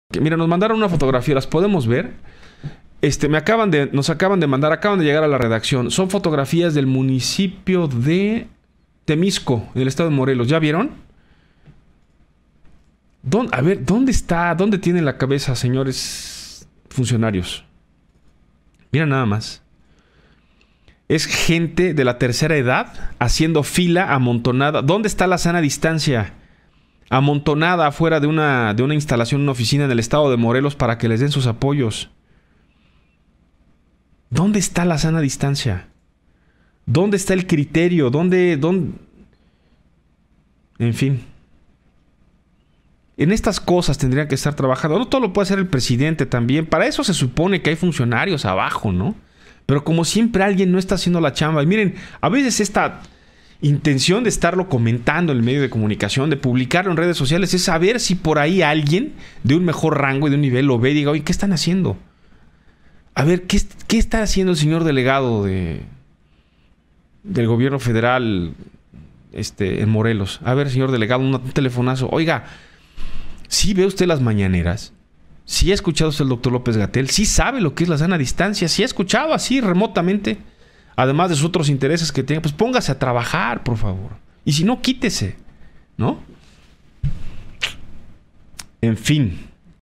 Personas de la tercera edad van por su pensión sin ningún operativo ante COVID19
En el municipio de Temixco, Morelos siguen entregando pensión a adultos mayores sin ningún operativo, reportan que muchos van enfermos a querer recibir su apoyo.